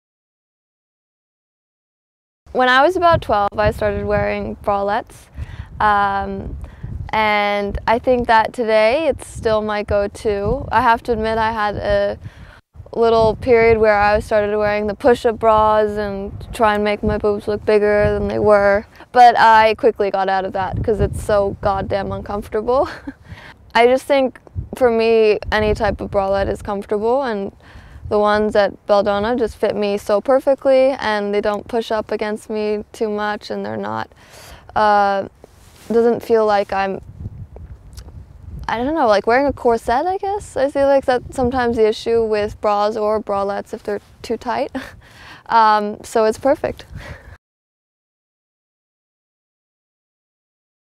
INSIDE BELDONA WITH SIF - BRA OR BRALETTE?
«I have to admit that I had a little period where I started to wear the push-up bras and try to make my boobs bigger than they were.»